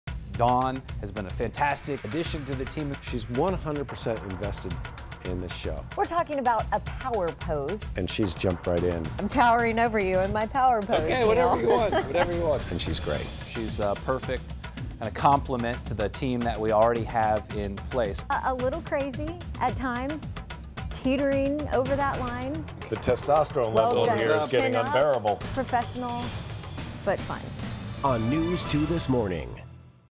A New Dawn
Dawn Davenport is the latest addition to the morning team on News 2. She is NO poser!